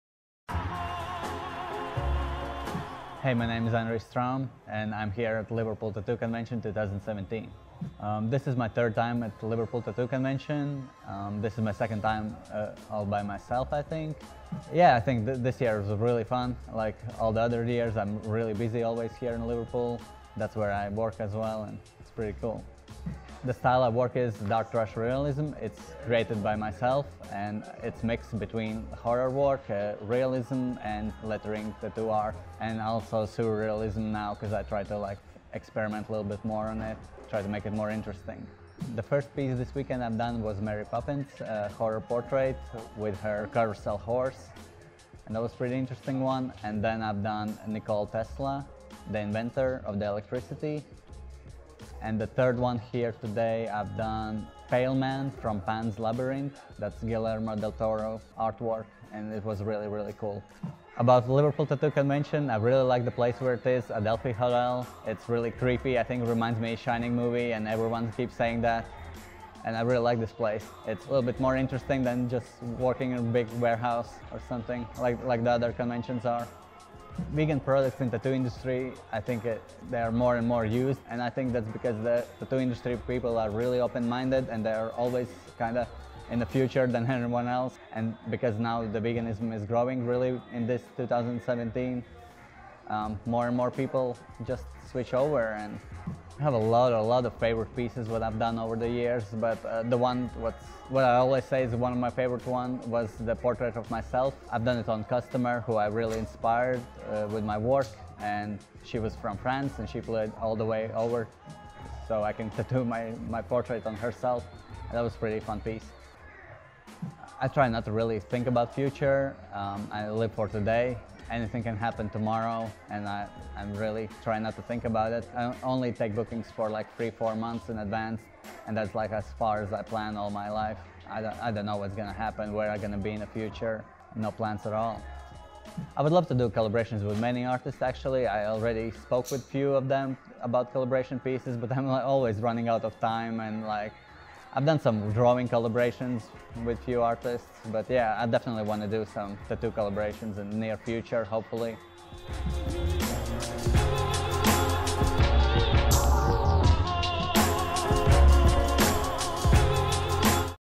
Anrijs Straume Interview | Liverpool Tattoo Convention 2017 | UK Tattoo Artist
Dark trash realism creator Anrijs Straume was at his hometown convention working on some killer pieces and gave us his thoughts on vegan tattoo products.

As well as interviewing Anrijs at the 2017 Liverpool Tattoo Convention, we were also lucky enough to get interviews with his tattooed wife, model & vegan ambassador Monami Frost and fellow tattoo artist Chris 51!

Read more about our interviews with Anrijs Straume, Monami Frost and Chris 51 at the 2017 Liverpool Tattoo Convention

Tattoo Artist: Anrijs Straume (Bold as Brass Tattoo Company)

Soundtrack: Brock Berrigan